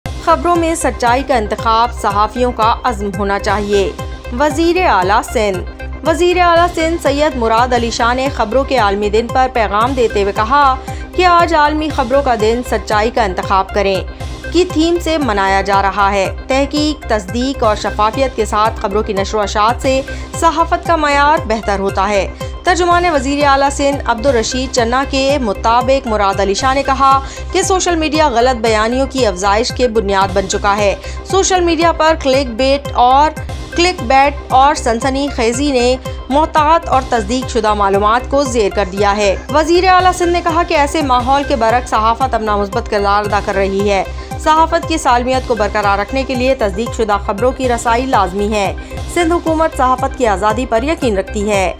"Choosing the Truth in News Should Be the Commitment of Journalists, Says Chief Minister Sindh"
"Choosing the Truth in News Should Be the Commitment of Journalists, Says Chief Minister Sindh"| SuchExpressNews Official

Sindh Chief Minister emphasized the responsibility of journalists to prioritize truth and integrity in their reporting. Speaking at a media event, he highlighted the critical role of the press in shaping public opinion and urged journalists to commit to ethical journalism, ensuring that truth is not compromised.

💬 "Journalists have a duty to report the truth, as it is the foundation of trust in society," says the Chief Minister.